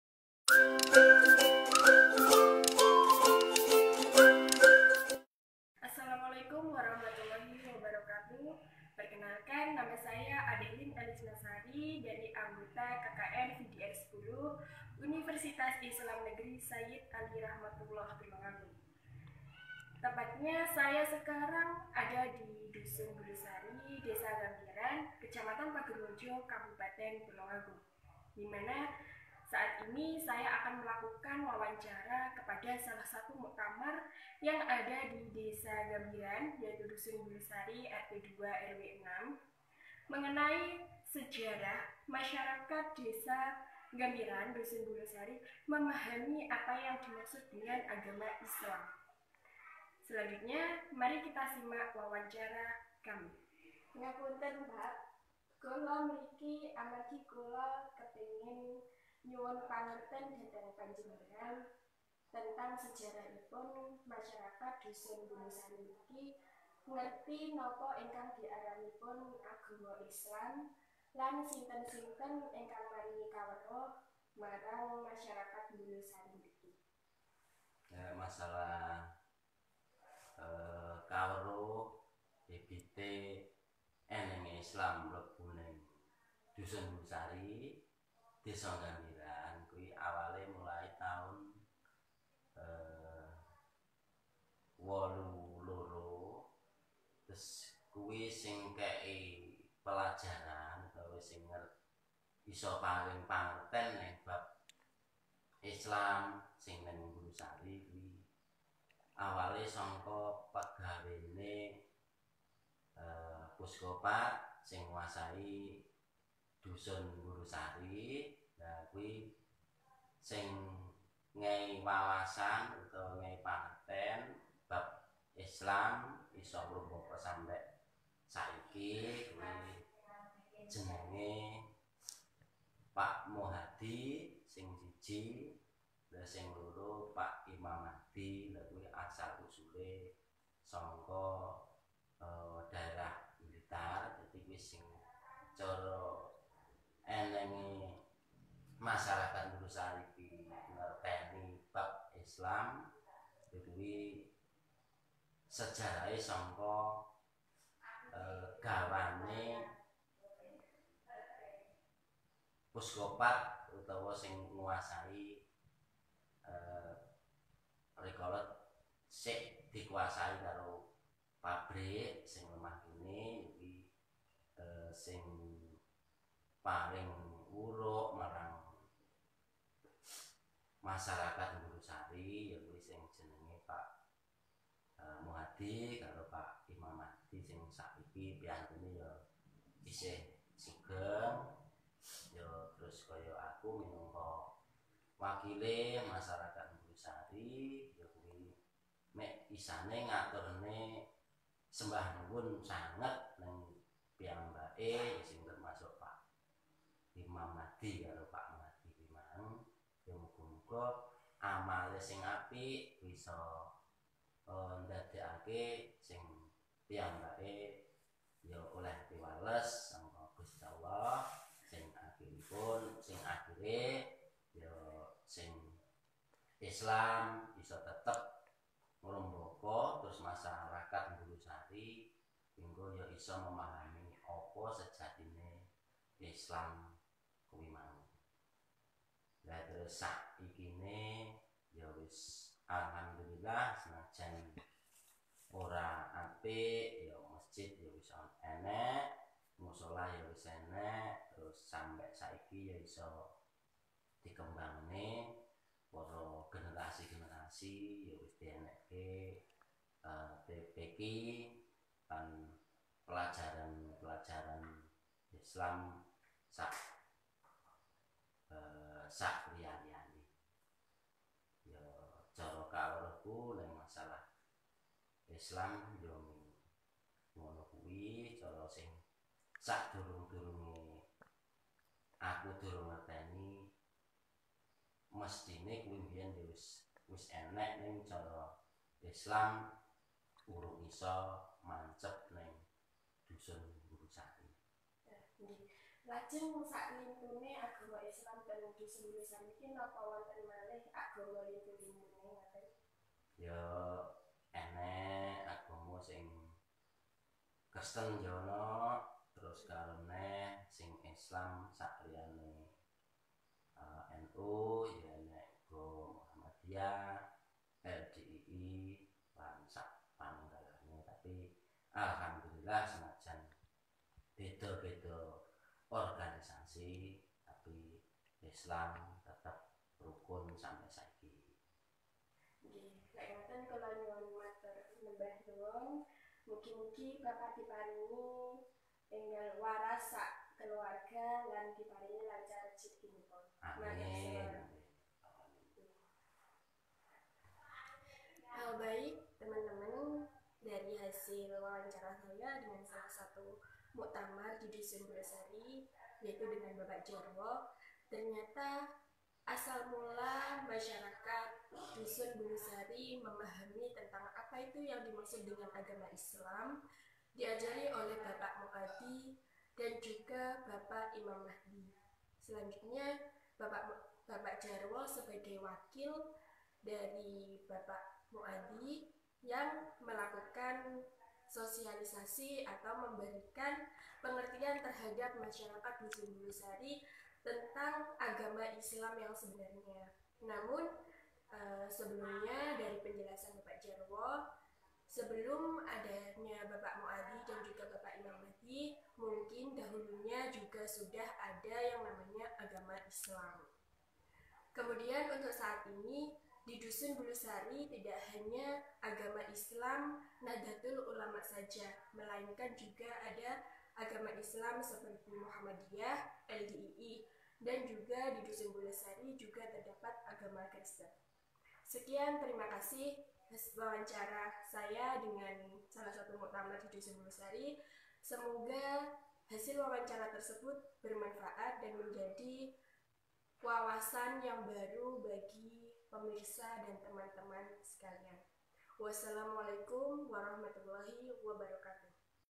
VIDEO MODERASI BERAGAMA KKN VDR 010|Gel II UIN SATU
assalamualaikum wr.wb
kami kirimkan video moderasi beragama dengan tema " Memahami apa yang dimaksud dengan Ajaran Agama Islam"
DPL : Arin Prajawinanti,S.Sos.,M.A.